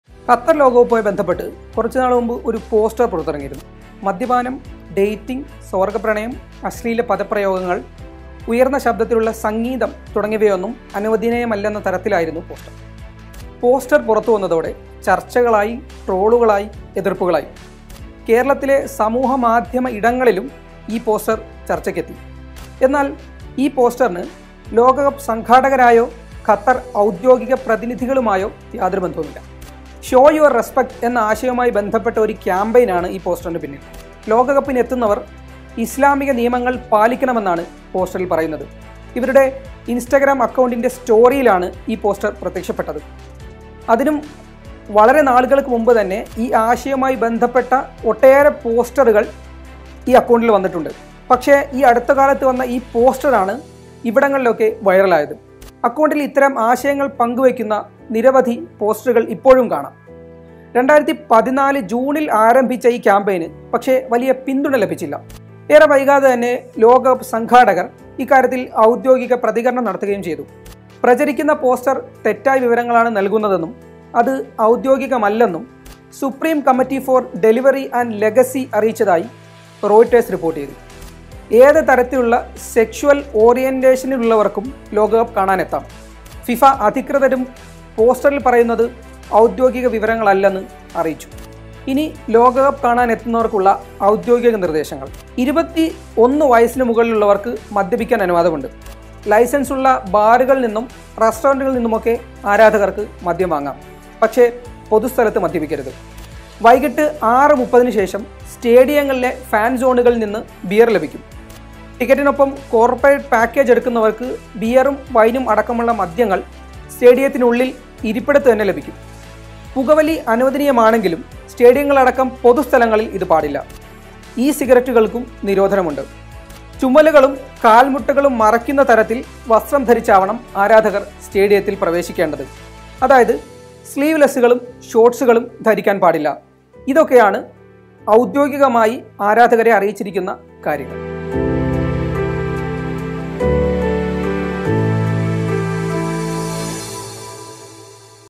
നവമാധ്യമങ്ങളിലെ വ്യാജപ്രചാരണങ്ങൾക്ക് ഖത്തറിന്റെ മറുപടി | Qatar World Cup 2022
ഖത്തർ ലോകകപ്പുമായി ബന്ധപ്പെട്ട് സമൂഹമാധ്യമങ്ങളിലൂടെ പ്രചരിച്ച പോസ്റ്ററിനെപ്പറ്റി അധികൃതർക്ക് പറയാനുള്ളത്